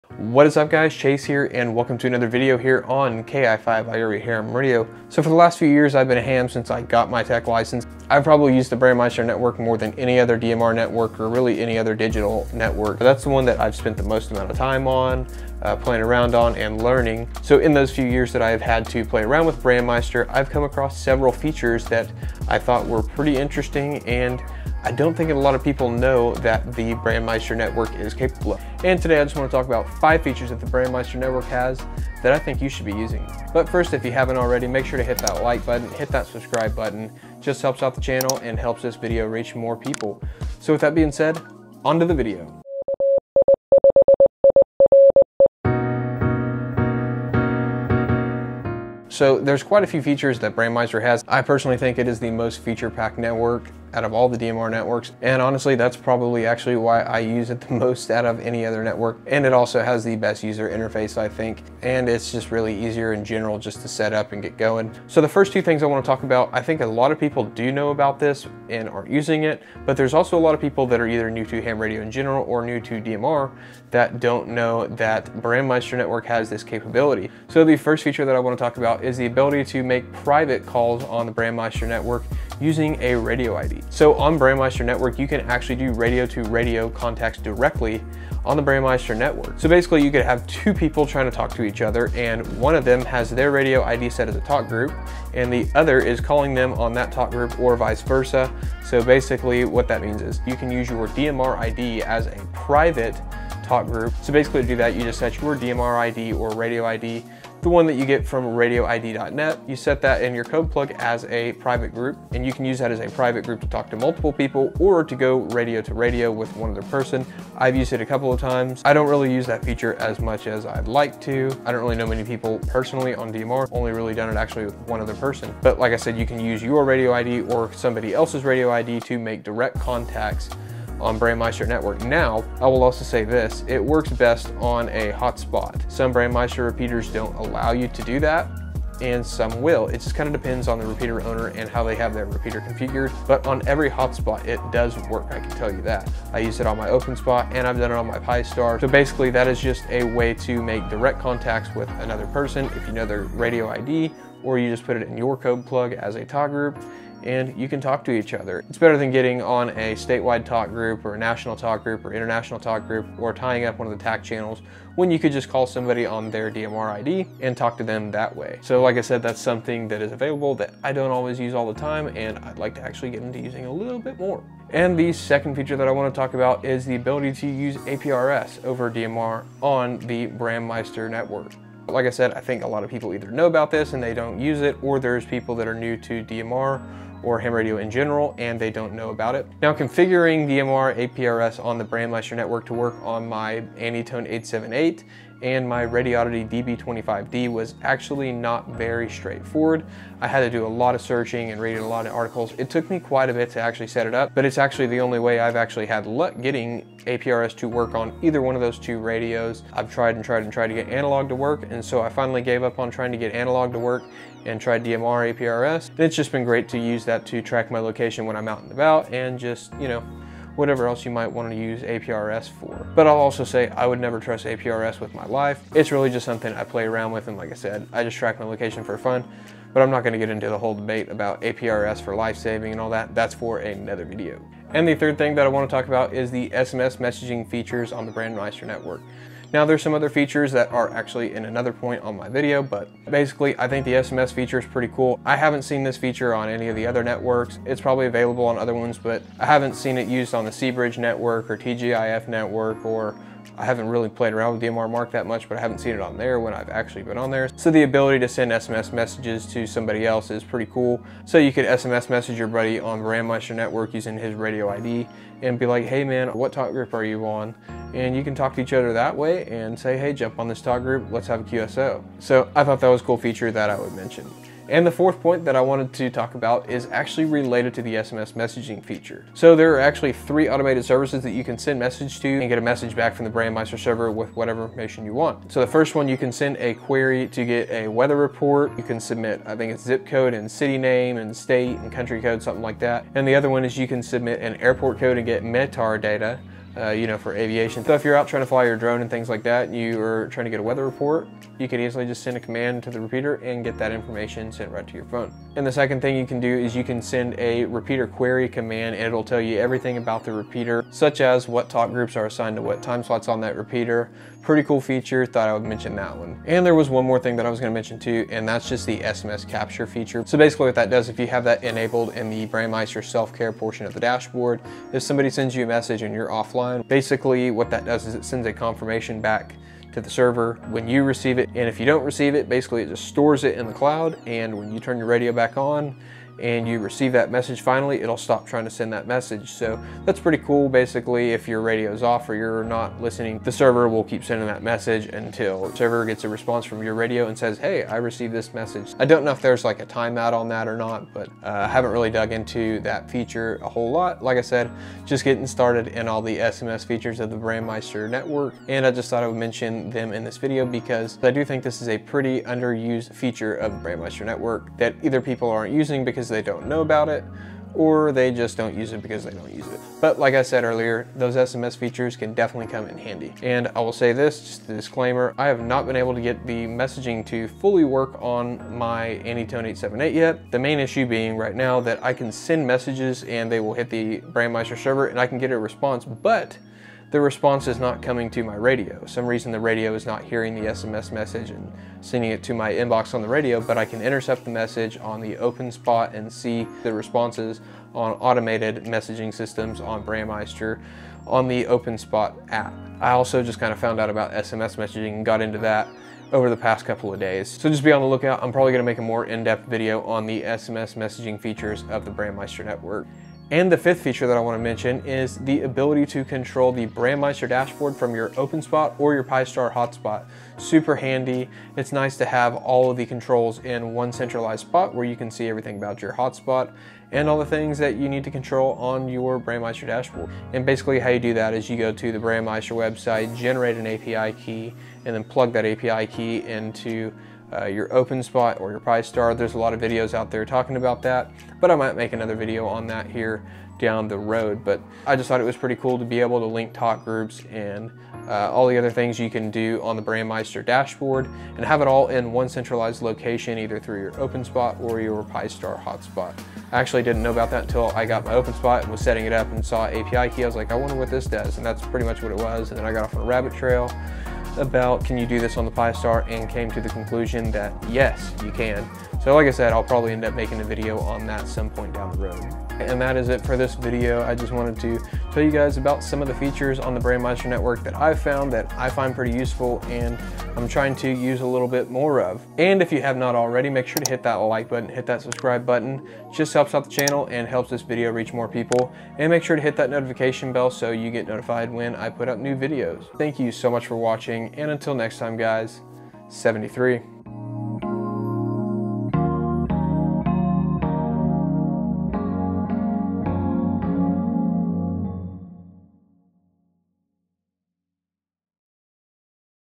5 BrandMeister DMR Features You Should Be Using!
It's been a while since I last made a DMR video!

Since I got my amateur radio Technician class license back in 2020, the main amateur radio DMR network I've found myself using in the BrandMeister ham radio DMR Network.

In this video, I'll talk about some of the features of the BrandMeister DMR Network that I think you should be using!

Let me know what BrandMeister features you like in the comments!

Use code "ki5irehamradio10" on qualifying purchases.

🌐The Ham Radio Logging Software I Use in the Shack (Only available for MacOS and iOS)